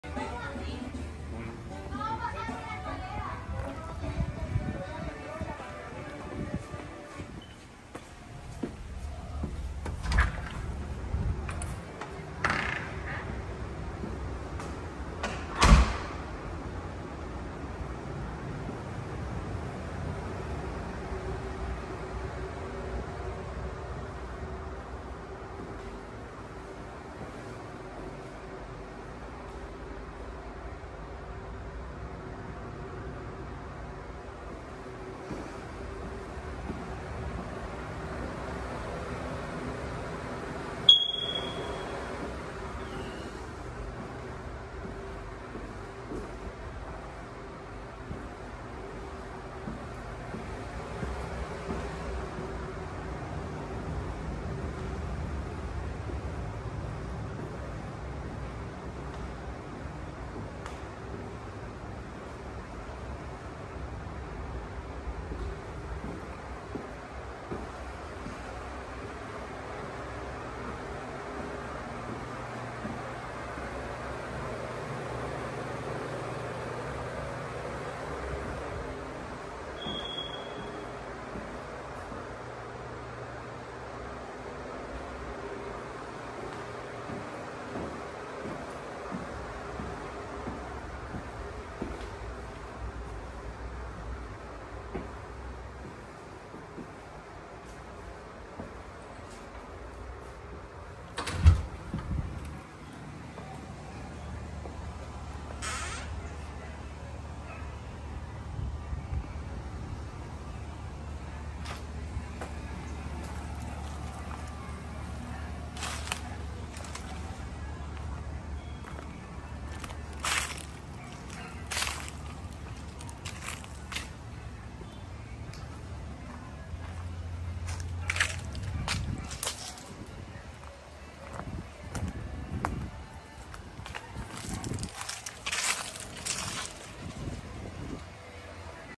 🏠 en Miami de 3 habitaciones, 2 baños, ventanas de impacto, NO HOA, techos y pisos nuevos
🏠 3 habitaciones, 2 baños, ventanas de impacto, NO HOA, techos y pisos nuevos, Tot SqFt: 1,173 y lote de Lot SF: 4,280 ✅ Disponible para financiamiento FHA 📍MIAMI, FL 33142 ✨ Precio $460,000 🔹 Real Estate Empire 📲 / @ventasyrentasmiami